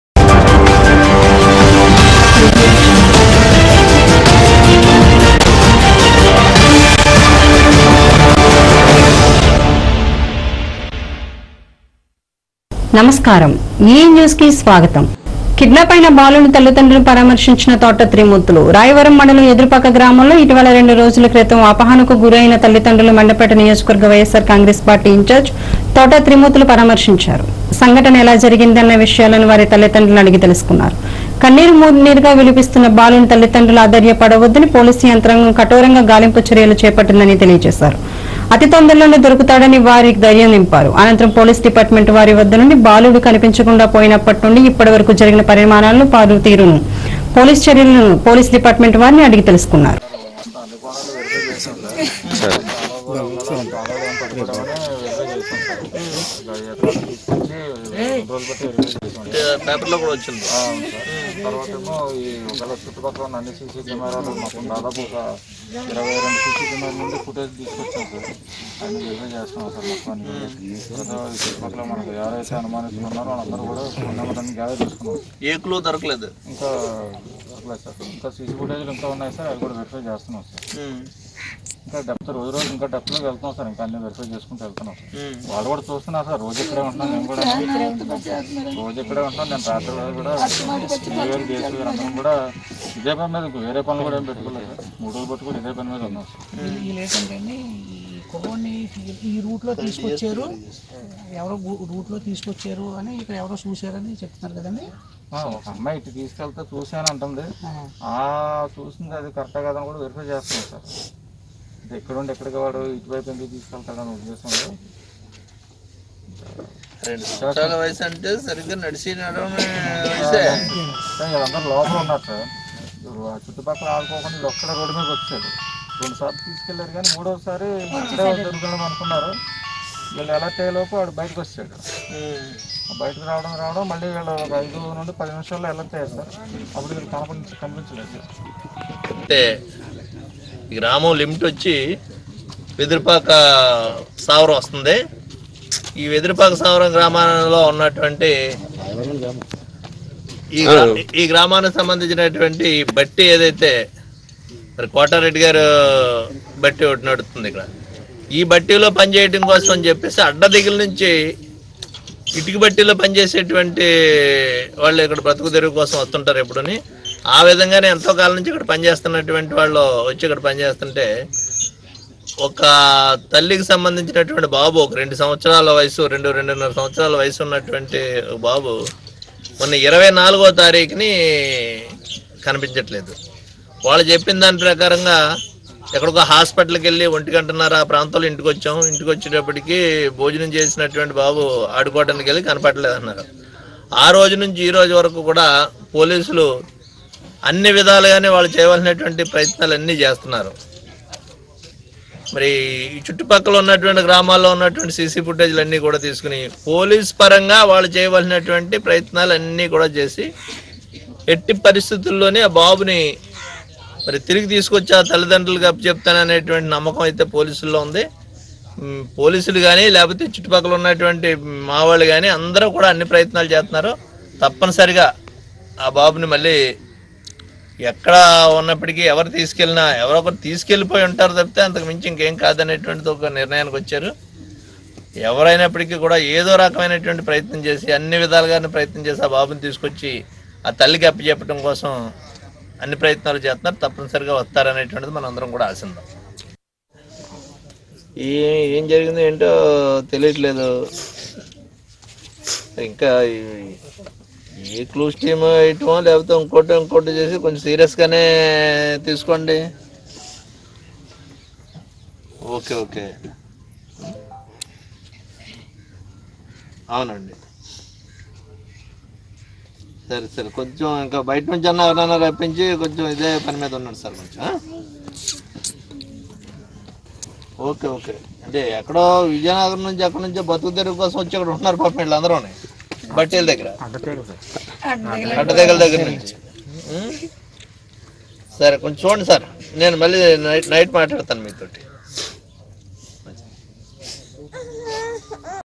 కిడ్నాపైన బాలిక తల్లిదండ్రుల పరామర్శించి కిడ్నాపైన తల్లిదండ్రులను పరామర్శించిన తోట త్రిమూర్తులు
కిడ్నాపైన బాలిక తల్లిదండ్రుల పరామర్శించి కిడ్నాపైన బాలుని తల్లిదండ్రులను పరామర్శించిన తోట త్రిమూర్తులు